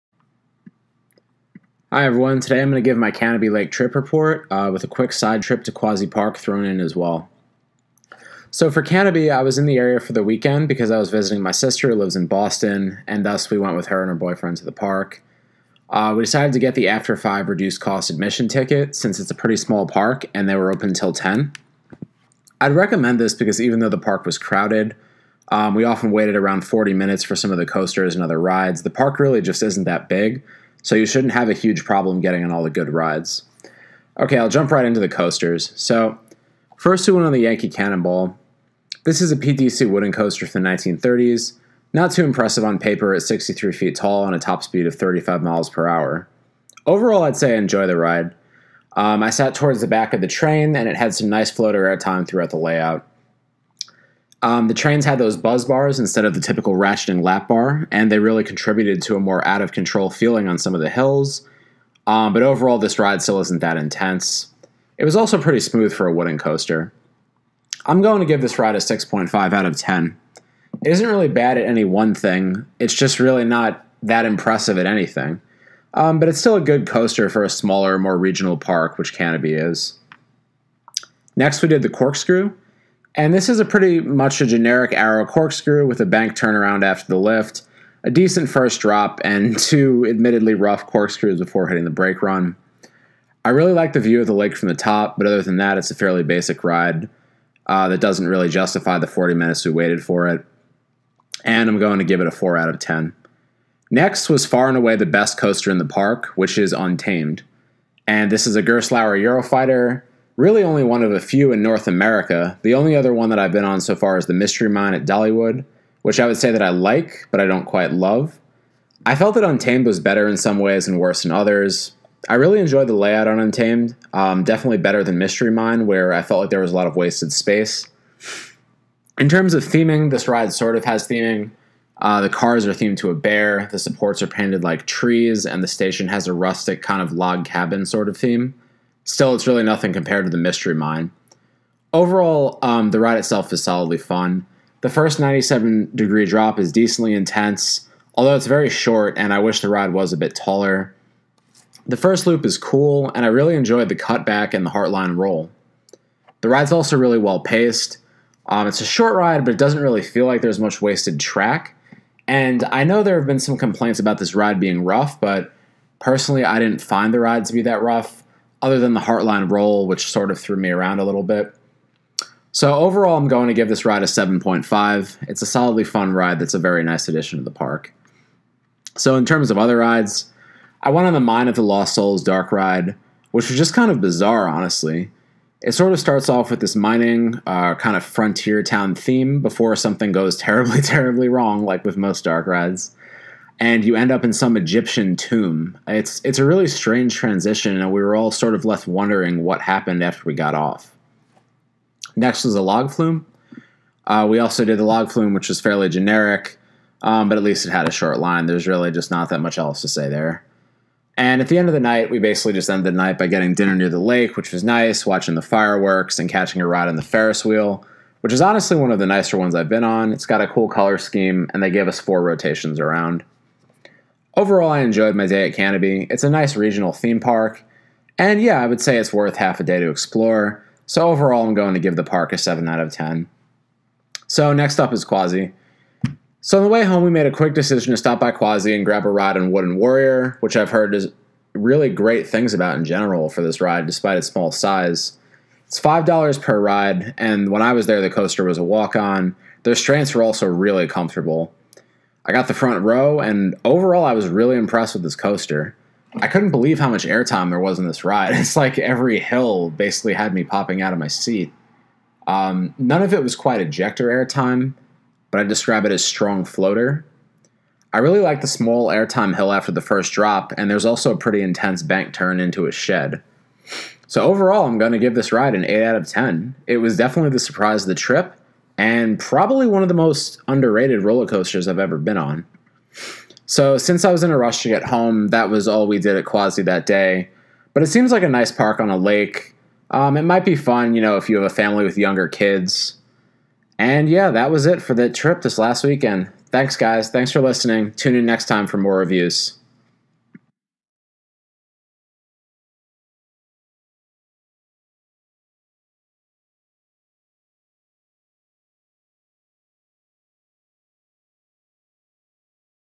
Canobie Lake Park: A Quaint New England Amusement Park with a Gerstlauer Euro-Fighter
Canobie Lake Trip Report with a quick side trip to Quassy to ride Wooden Warrior.